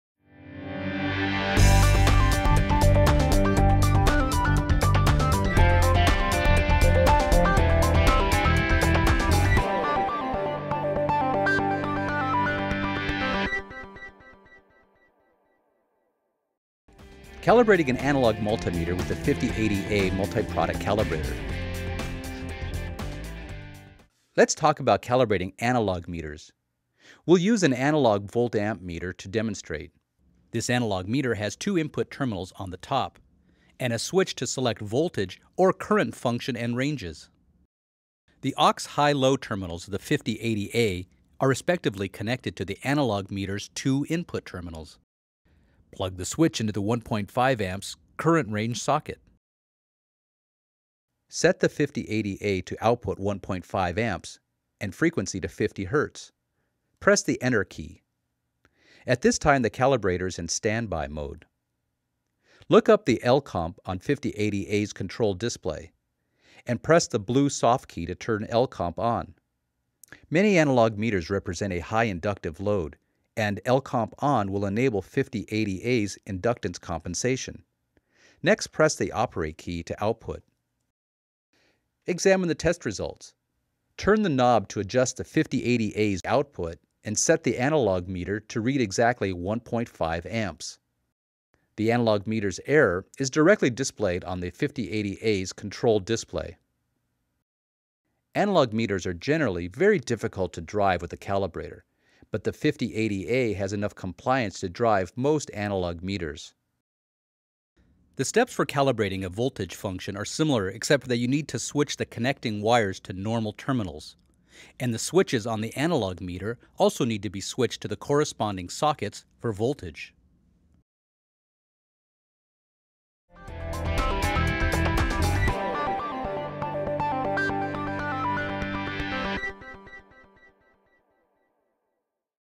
The 5080A Multi-Product Calibrator: Calibrating an Analog Multimeter
Using the 5080A Multi-Product Calibrator to calibrate an analog multimeter. Step-by-step demonstration.